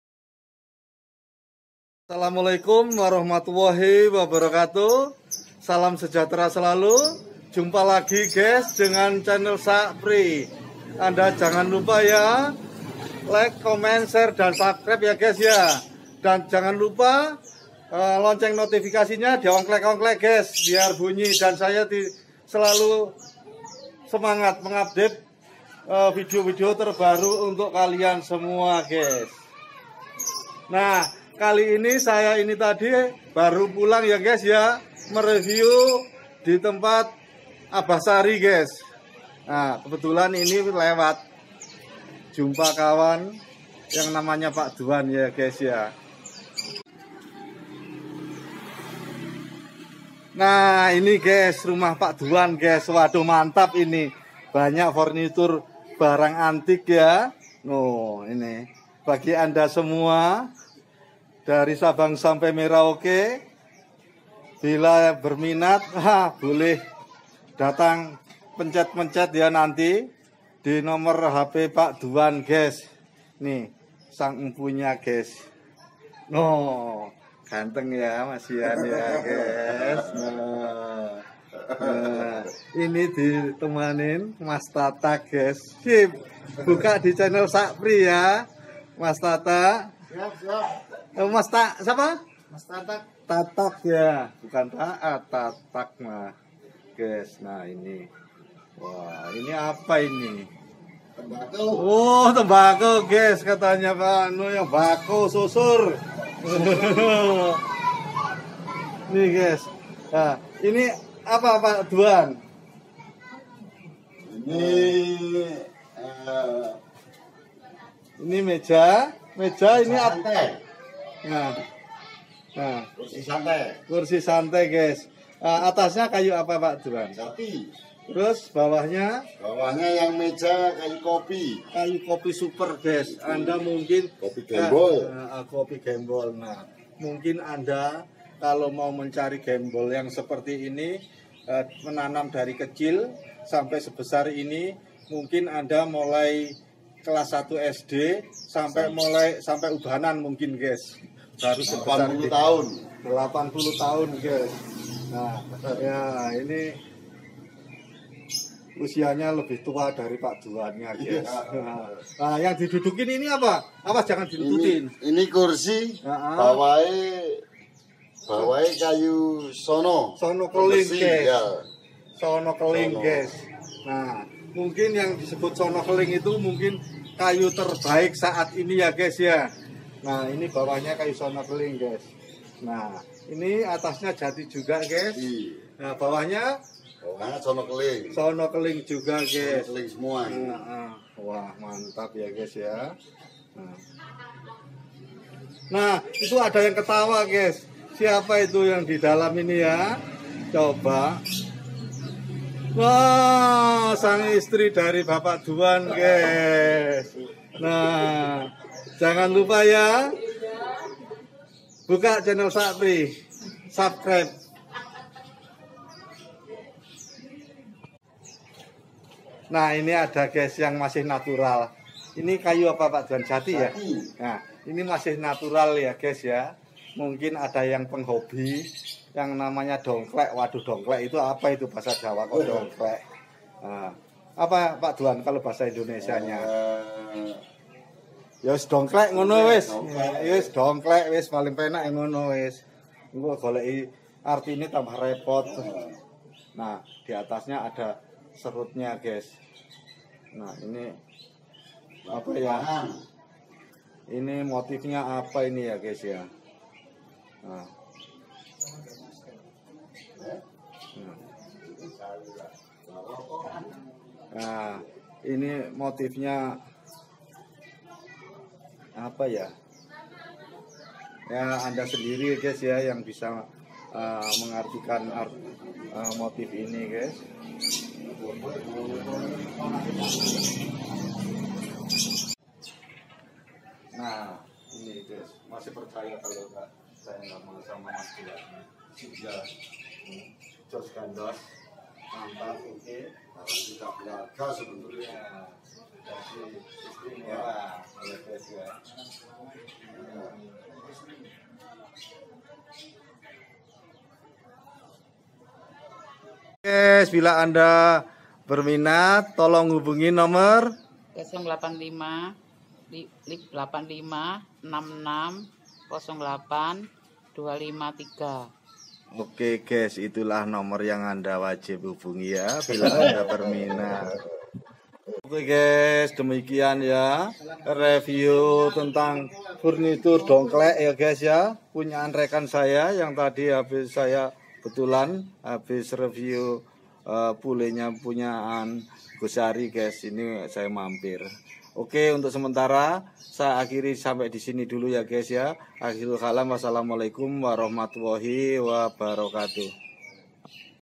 Meja Donglek kayu jati kaki Sono Keling mantap
Meja santai Donglek kayu jati berkaki Donglek kayu sonokeling mantap jos gandos.